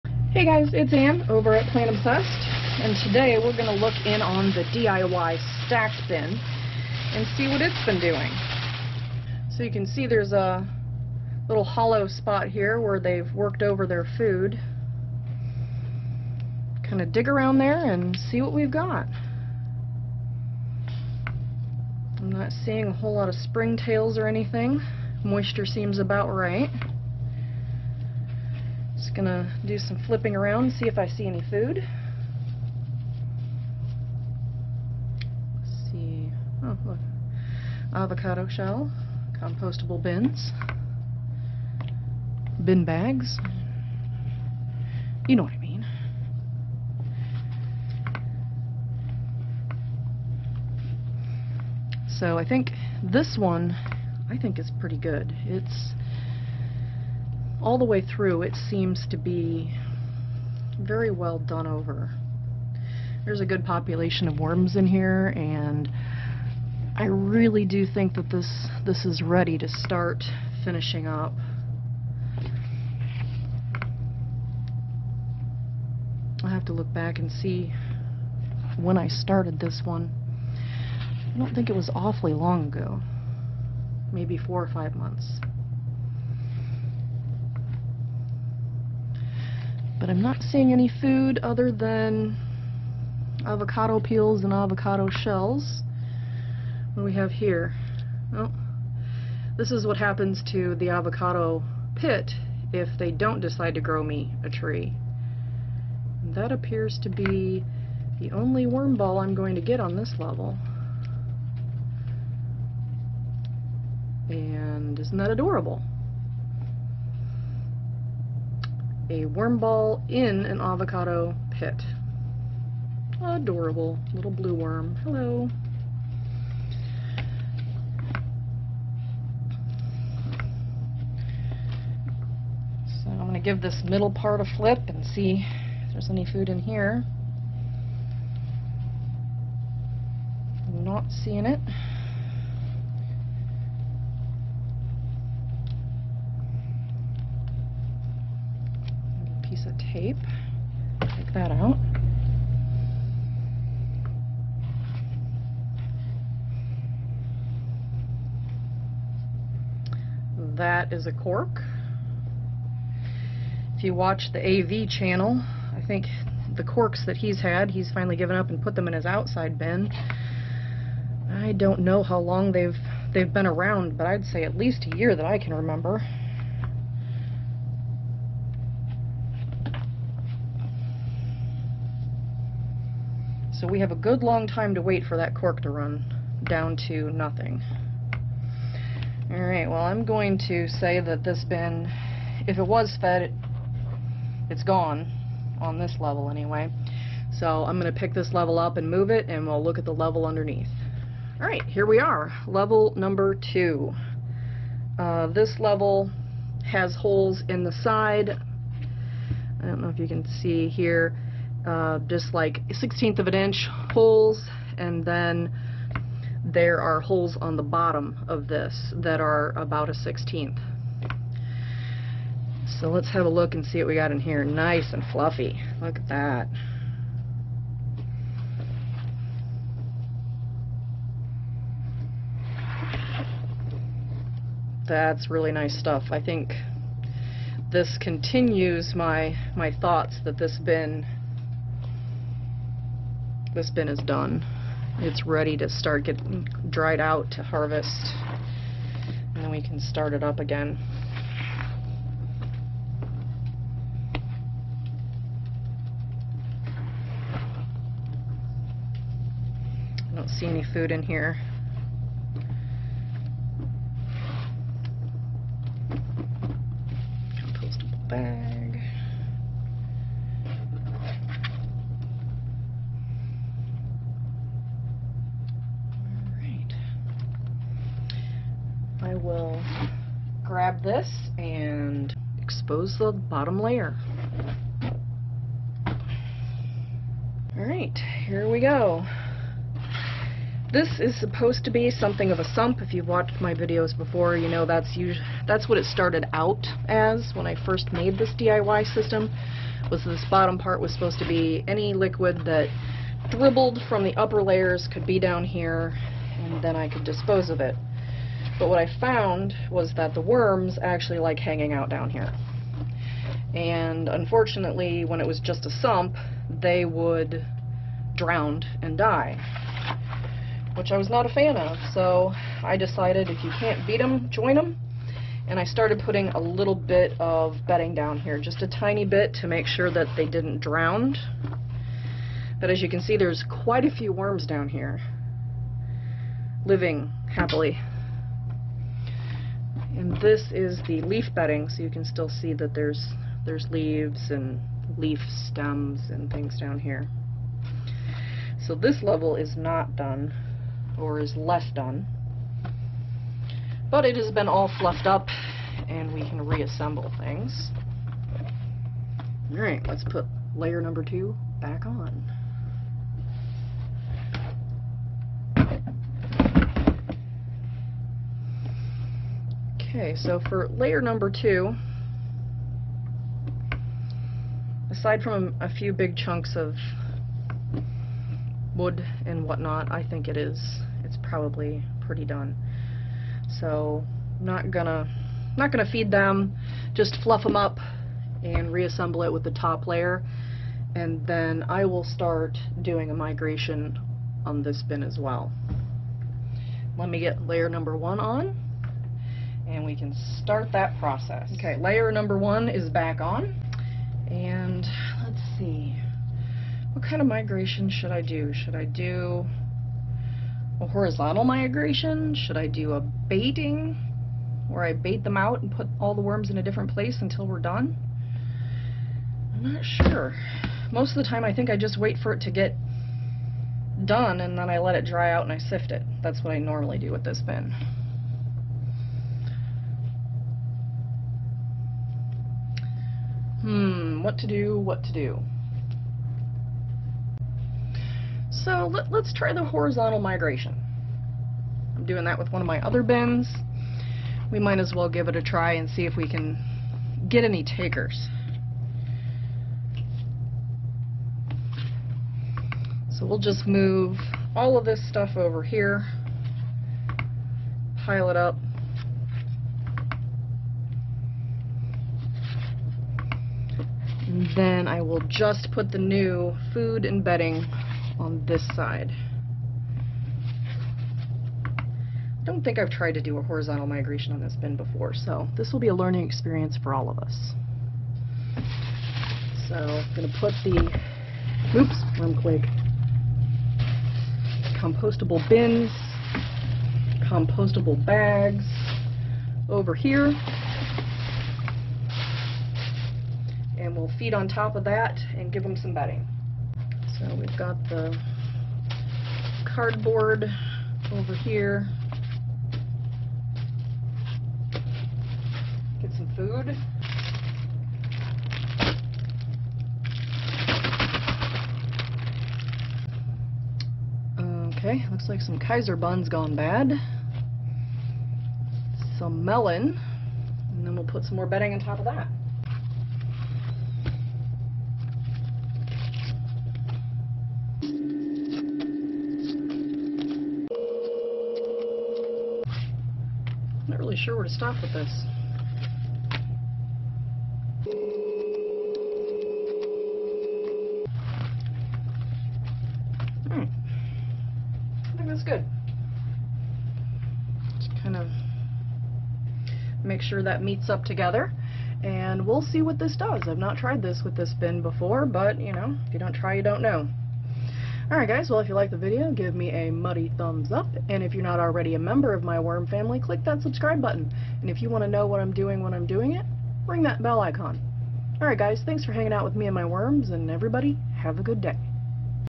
STACKED DIY BIN -SPEEDING THROUGH SUMMER
Looking in on the DIY stacked worm bin.  They are processing faster than usual. Is it that it is summer?